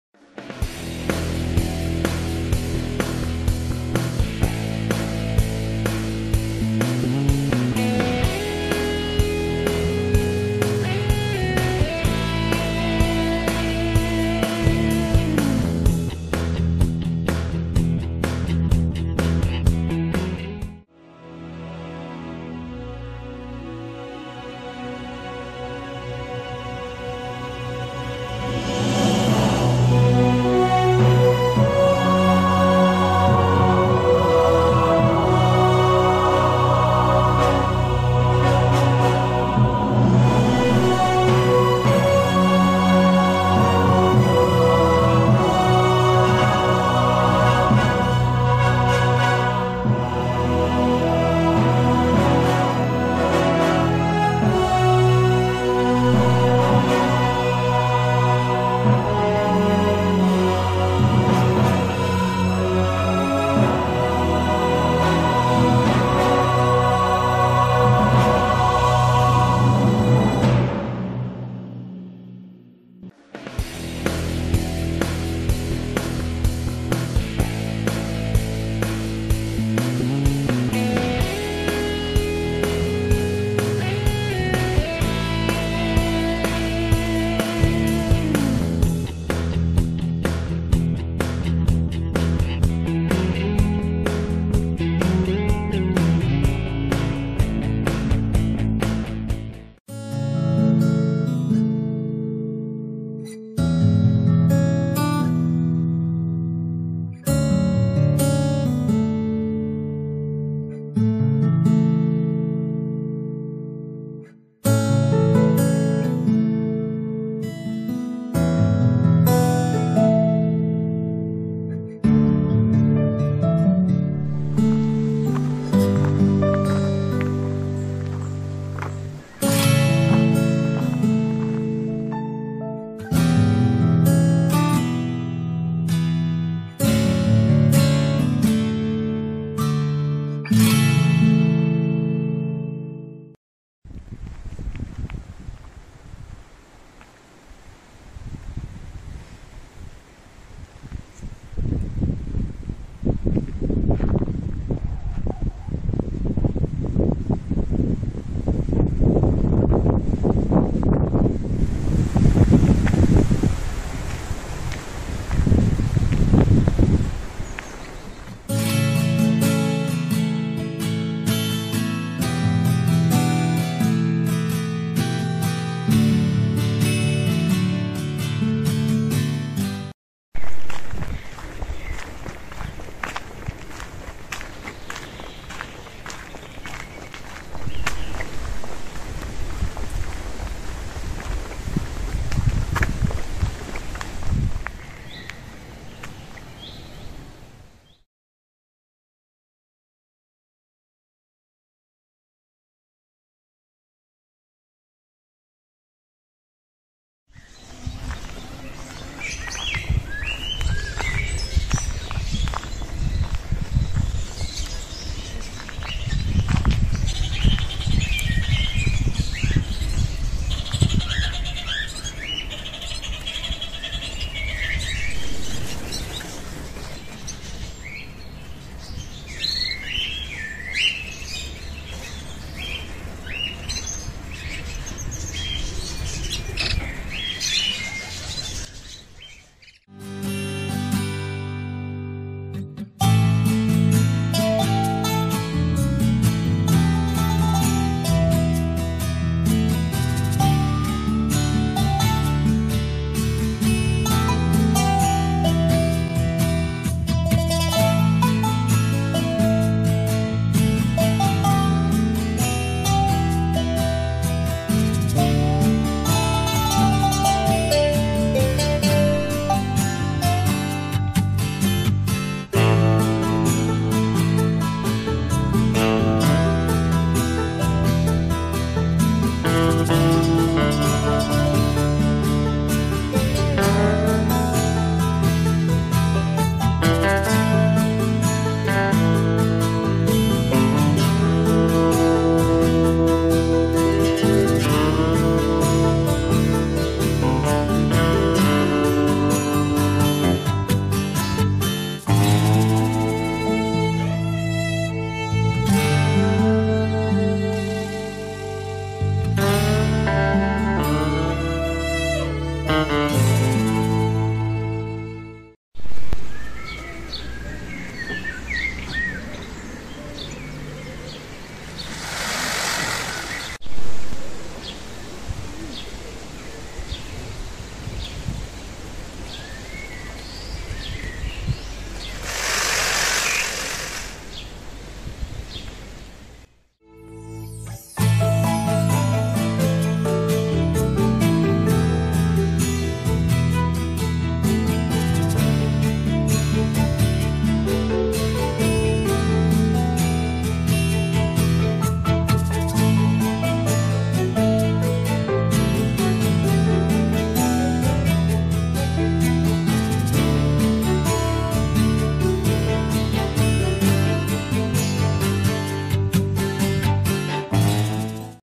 Wandern um den Höxberg - Erlebnispark, Windmühle und Soestwarte(2021, 4k)
Nach einer kurzen Erkundung des alten Stadtkerns von Beckums mit der St. Stephanus ging es mit dem Auto zum Erlebnispark Höxberg zu einer abendlichen Rundwanderung durch den Buchenwald zum Aussichtspunkt im Osten und um die vielfältigen Gehege des Naturparks zur Windmühle(erbaut 1853) am Höxberg. Dann weiter um die Gehege zur Soestwarte(erstmals erwähnt 1464) und bei Sonnenuntergang wieder zurück zum Startpunkt. Entfernung ca. 4 km, Dauer ca. 1 Std. und +/-60 Höhenmeter. GPS-Daten